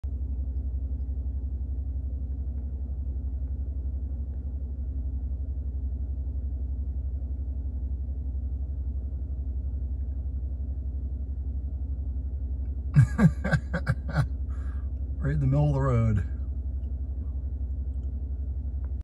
Baby deer fawn feeding
Baby deer goes under mamma deer and starts feeding in the middle of the road.